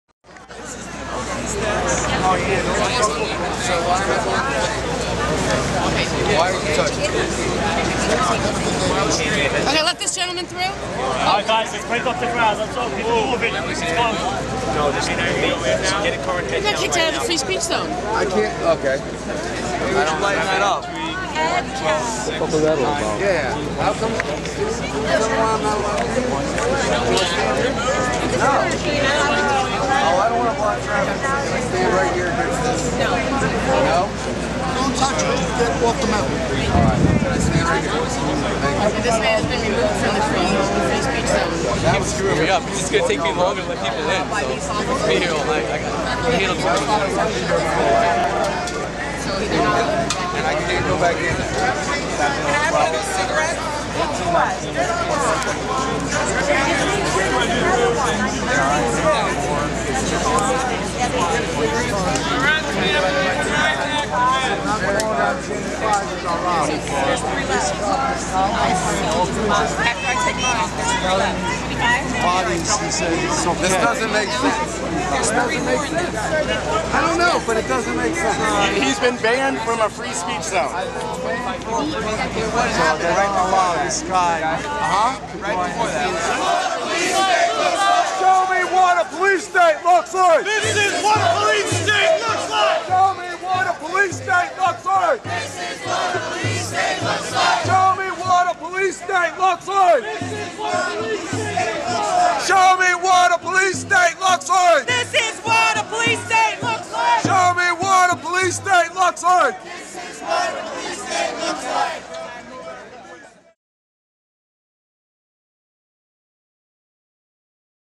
OWS A20 - THIS IS WHAT A POLICE STATE LOOKS LIKE
PARK POLICE & SWAT TEAM TELL PROTESTER TO GET OUT OF THE ( FREE SPEECH ZONE ) THOUGHT AMERICA WAS FREE.